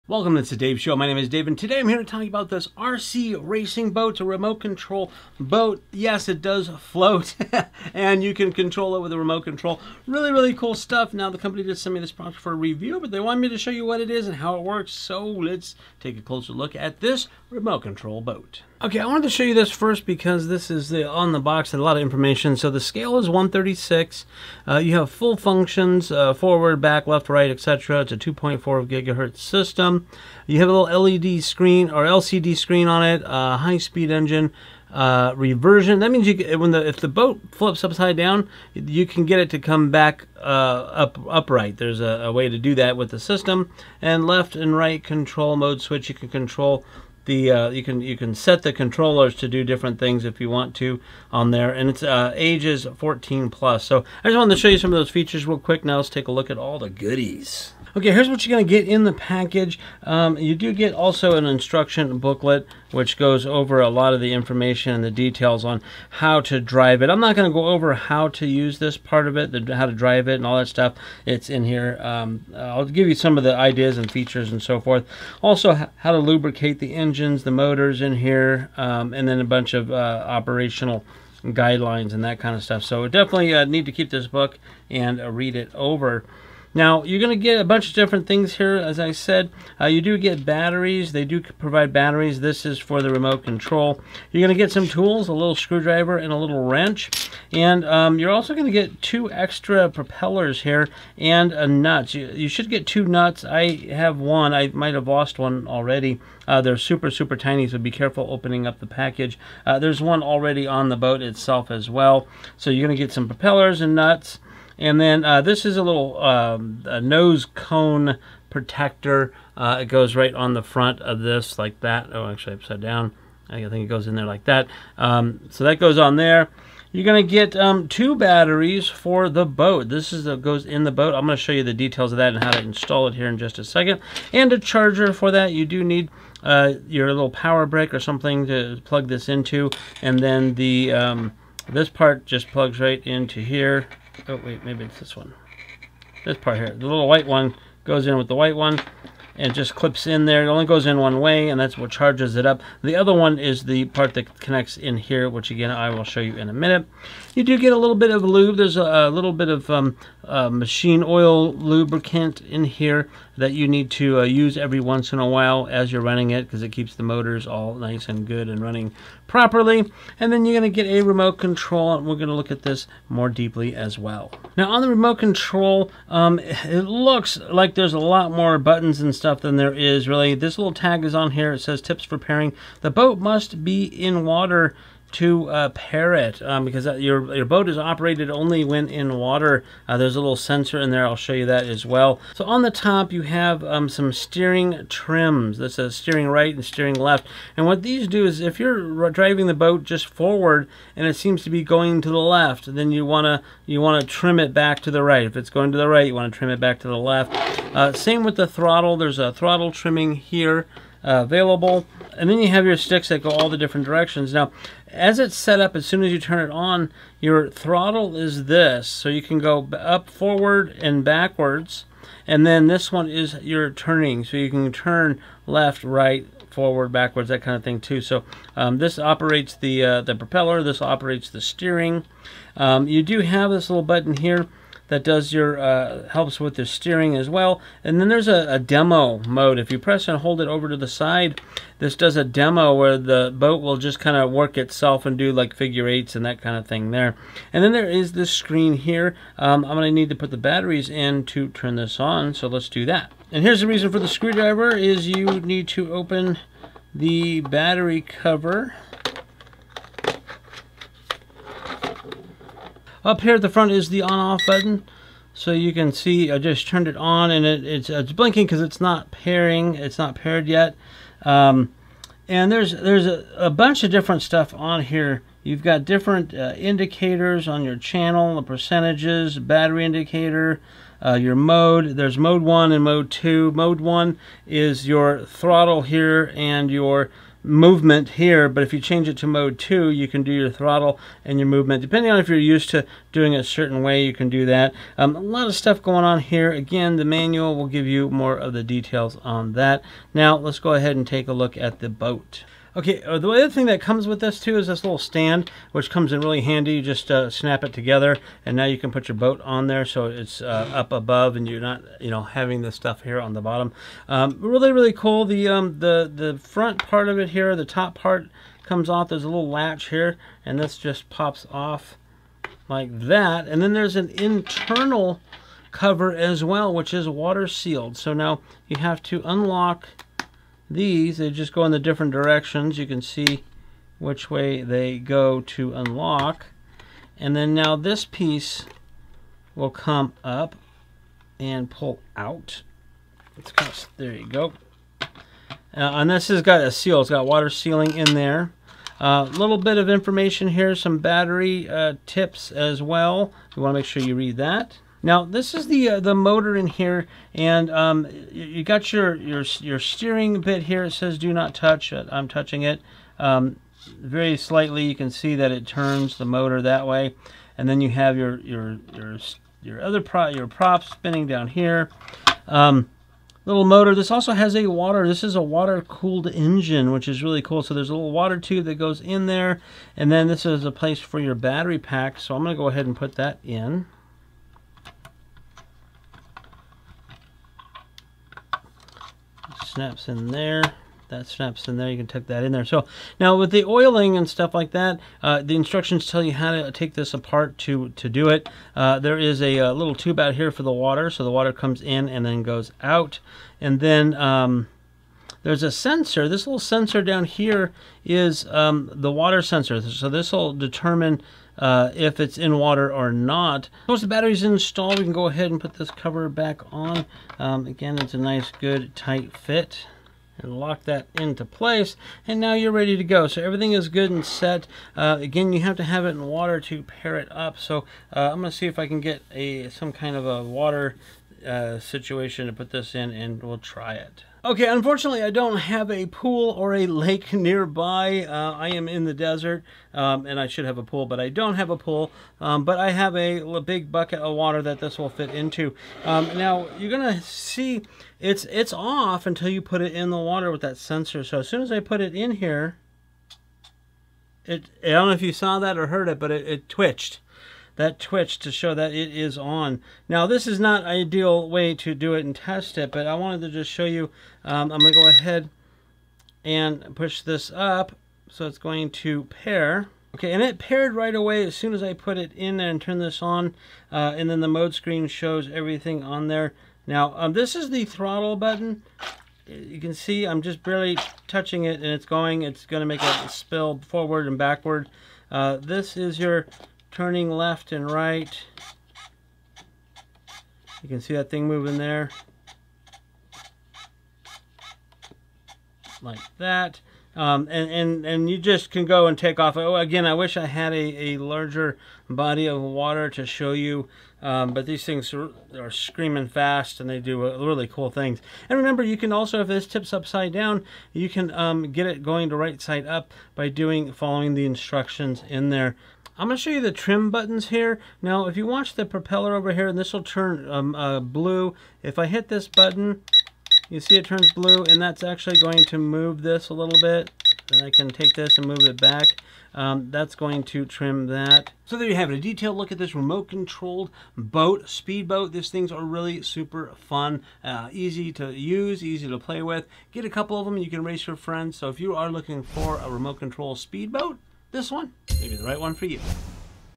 Remote Control Boat for Pools and Lakes
Zyerch RC Boat - Remote Control Boat for Pools and Lakes, 25 km/h Fast RC Boats for Adults and Kids, 2.4Ghz Self-Righting Racing Boats with 2 Rechargeable Battery, Low Battery Alarm,